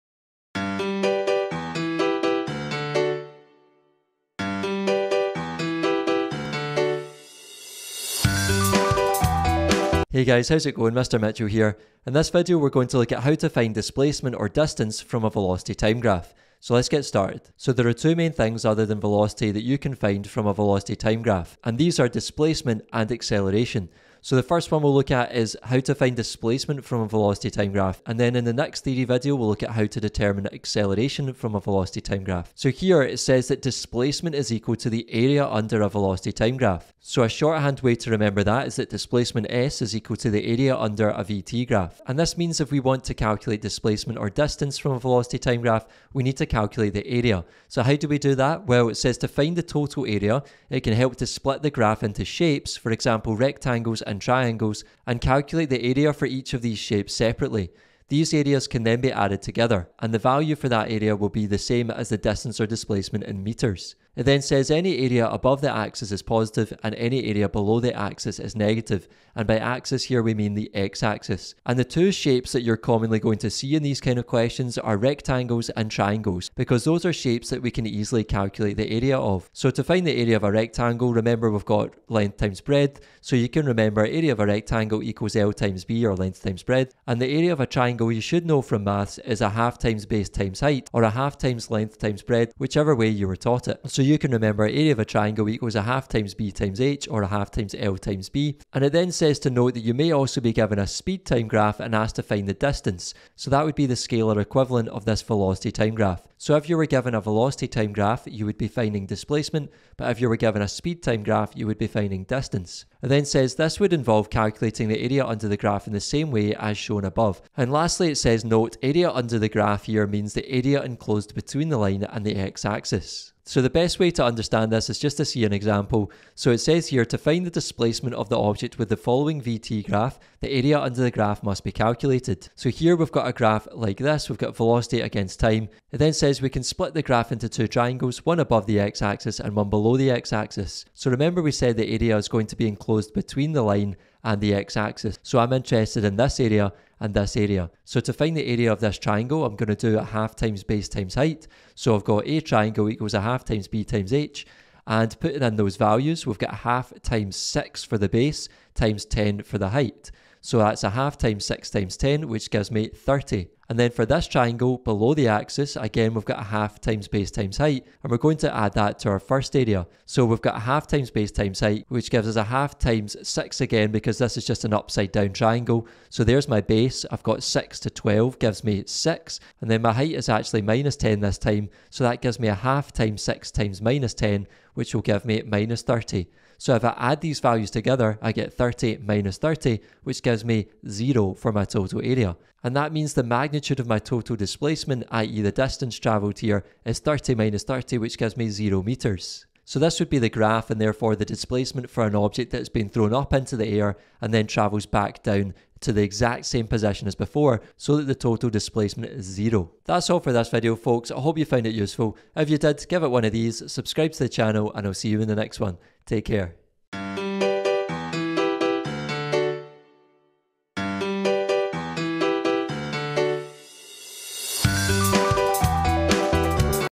National 5 Physics | Dynamics | Finding Displacement from a Velocity -Time Graph | THEORY
A brief overview of how to find distance/displacement from a velocity-time graph from the Dynamics topic in the National 5 Physics course.